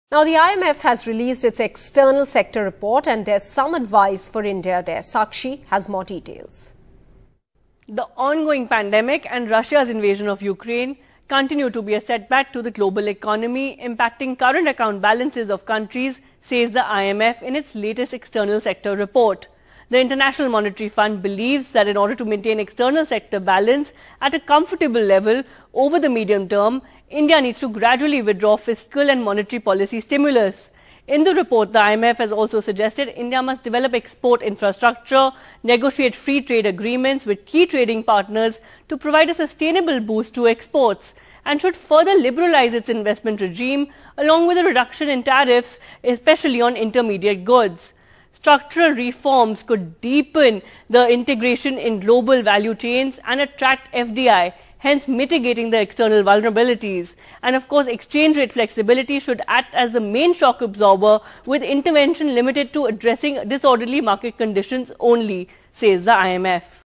India Should Gradually Withdraw Fiscal, Monetary Policy Stimulus, Recommends IMF
To maintain external sector balance at a comfortable level over the medium term, the International Monetary Fund has recommended that India should gradually withdraw its fiscal and monetary policy stimulus. Sakshi Bajaj reports.